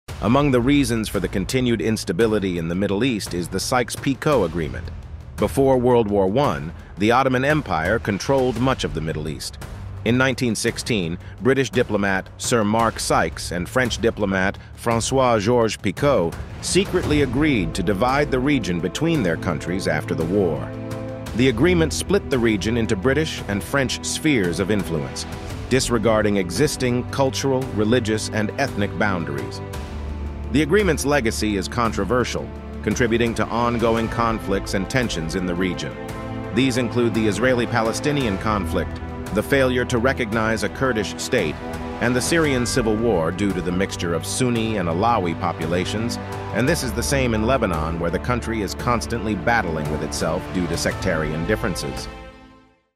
Middle East Conflicts traced back to Sykes Picot Agreement.
Among the reasons for the continued instability in the Middle East is the Sykes-Picot agreement. Before World War I, the Ottoman Empire controlled much of the Middle East.  In 1916, British diplomat Sir Mark Sykes and French diplomat François Georges-Picot secretly agreed to divide the region between their countries after the war.

The agreement split the region into British and French spheres of influence, disregarding existing cultural, religious, and ethnic boundaries  The agreement's legacy is controversial, contributing to ongoing conflicts and tensions in the region.

These include the Israeli-Palestinian conflict, the failure to recognize a Kurdish state, and the Syrian Civil War due to the mixture of Sunni and Alawi populations and this is the same in Lebanon where the country is constantly battling with itself due to sectarian differences.